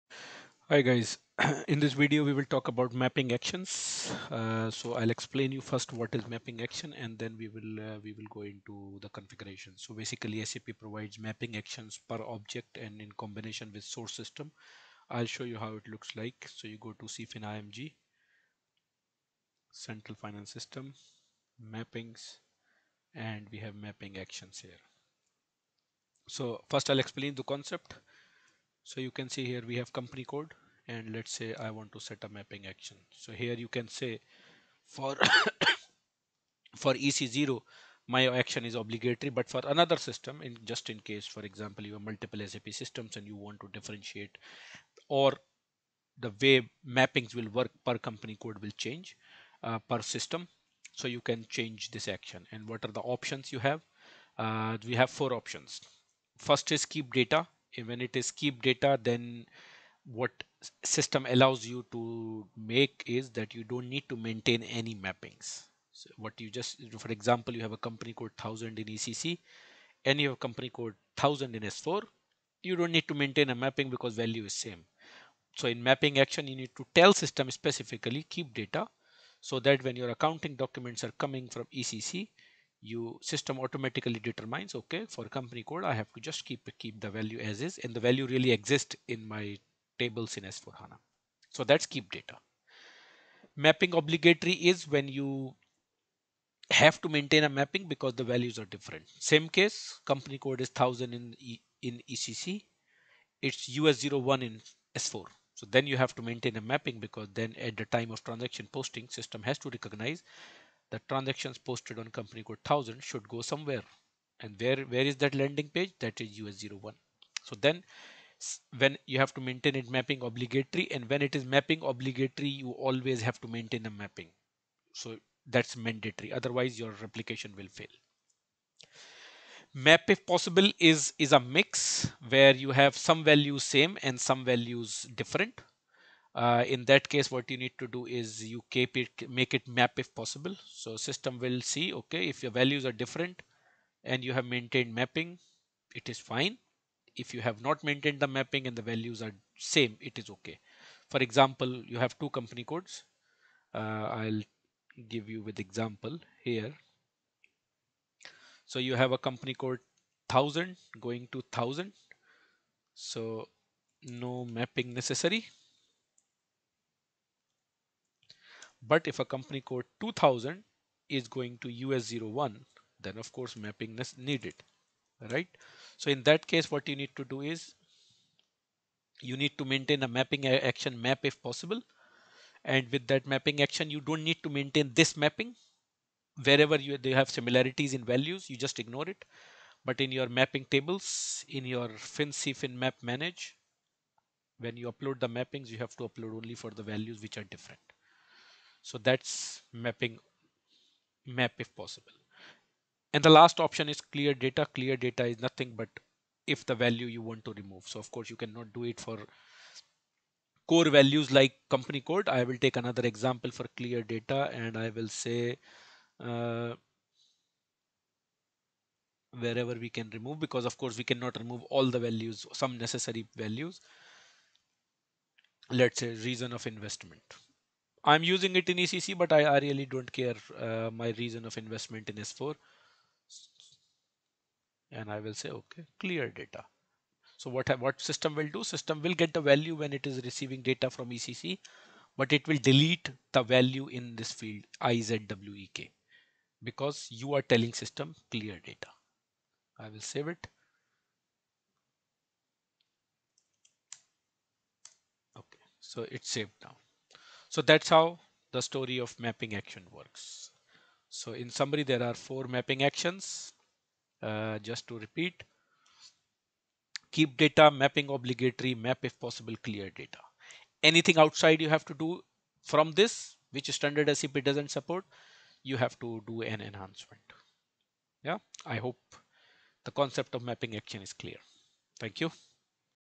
Mapping Actions in SAP Central Finance
In this video we have discussed the concept of 4 Mappings actions provided by SAP in CFIN
 - Keep Data
 - Mapping Obligatory
 - Clear Data
 - Map if Possible